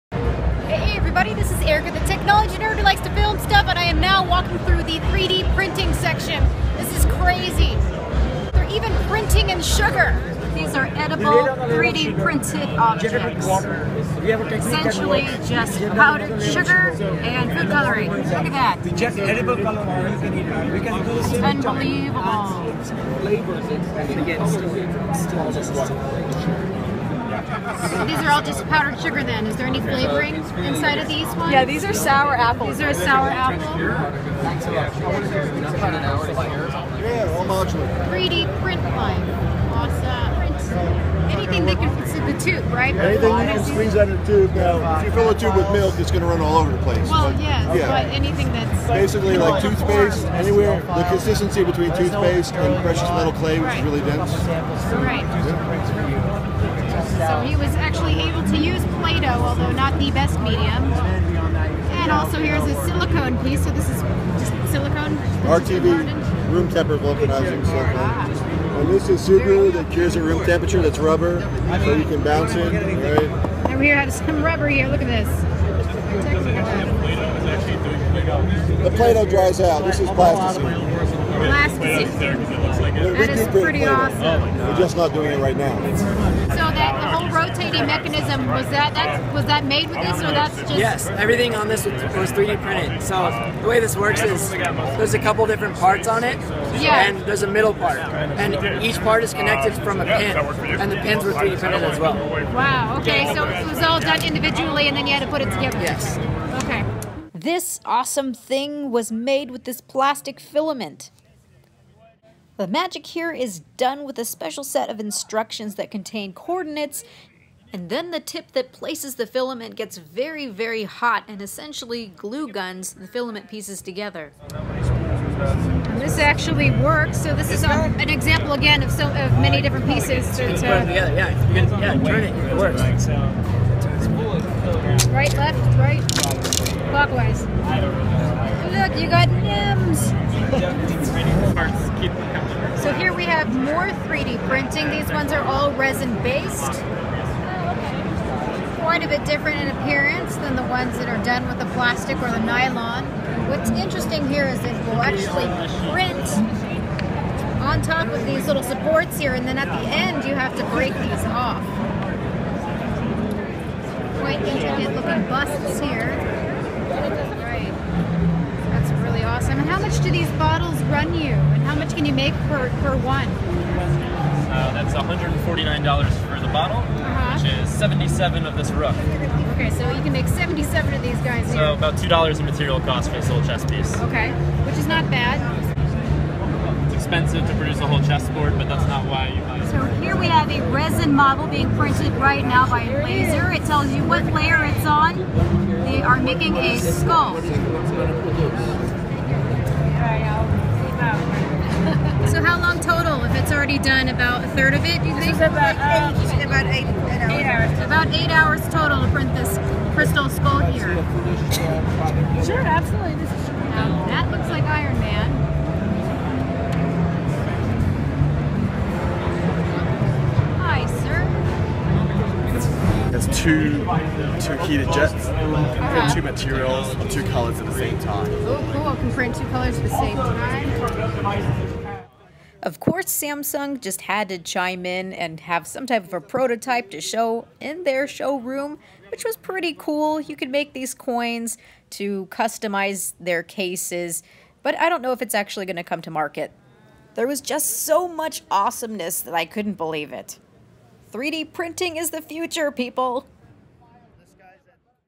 CES 2014: 3D Printing Awesomeness!
I saw many different kinds of 3D printers! One even printing sugary treats. Check it out!